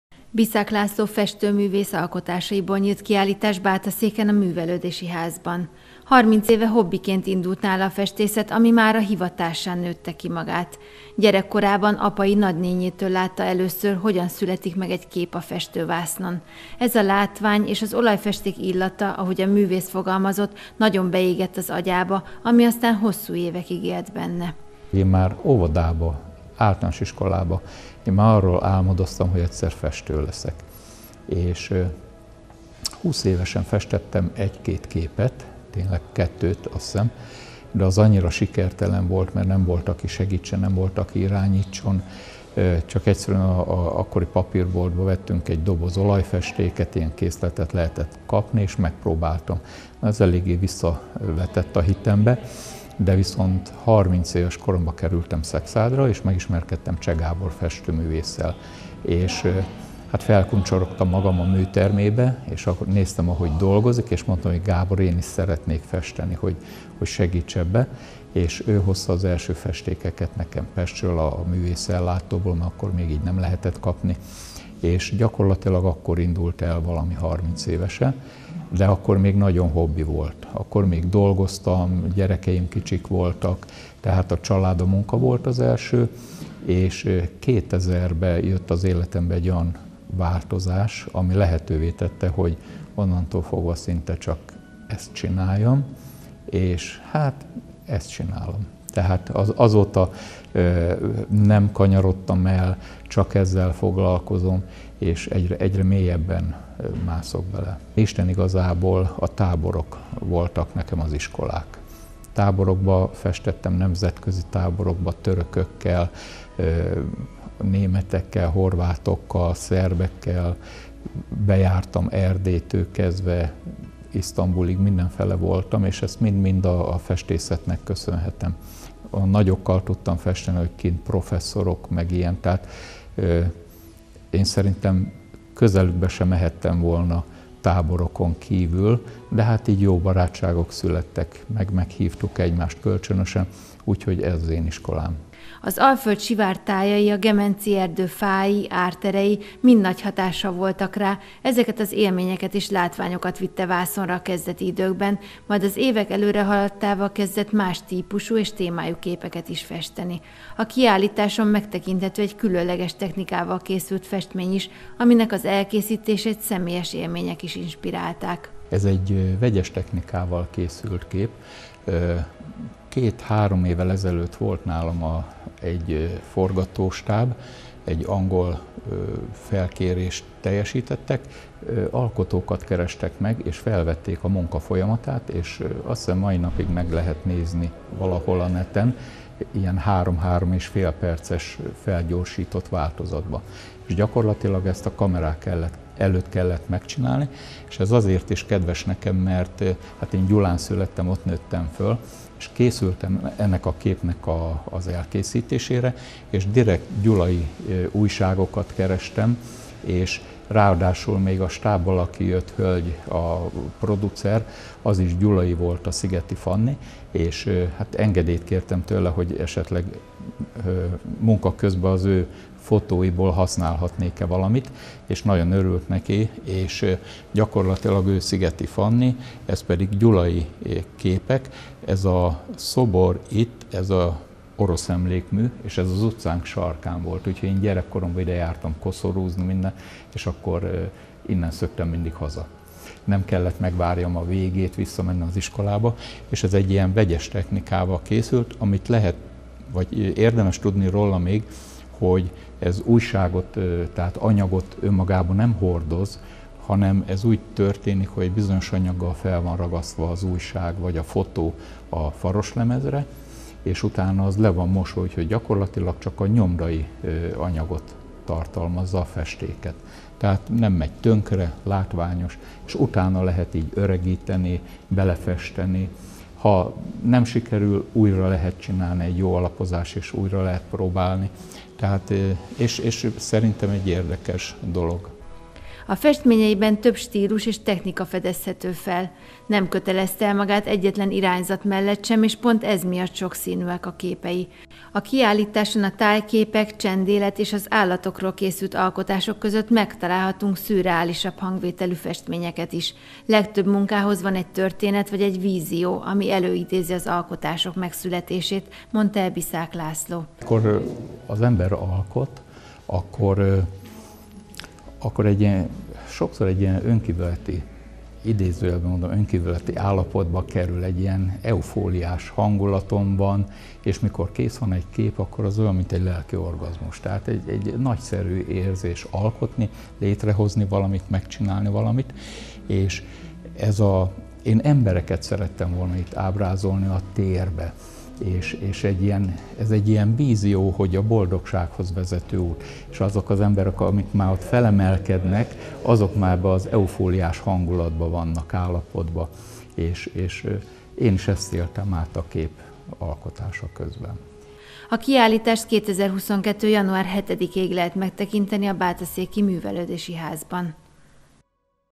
Biszák László festménykiállítása Bátaszéken
Biszák László festményeit nézhetik meg az érdeklődők, ha ellátogatnak a bátaszéki Művelődési Házba. A hobbinak indult festészet mára hivatássá vált a festőművész számára. A képek tematikájukat tekintve sokszínűek, változatosak, a tájképektől kezdve, a szürreális témájú alkotásokig. A kiállítást 2022. január 7-ig lehet megtekinteni.